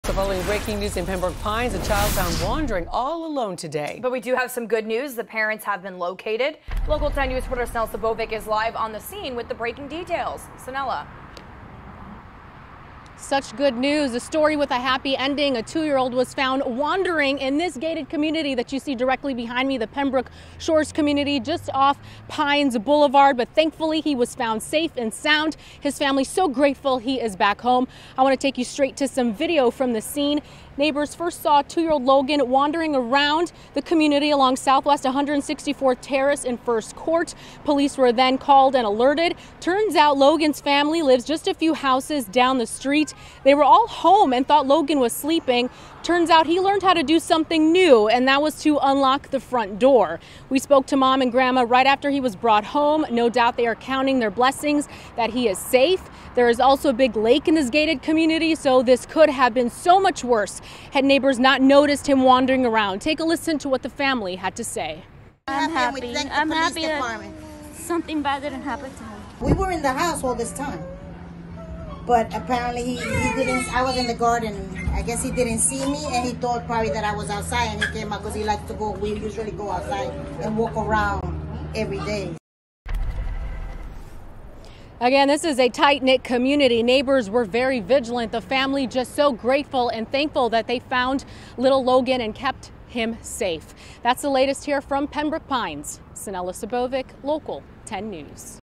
Boy reunited with parents after being found alone in Pembroke Pines
Pembroke Pines police confirmed that they have found the parents of a young boy who was discovered alone Tuesday morning.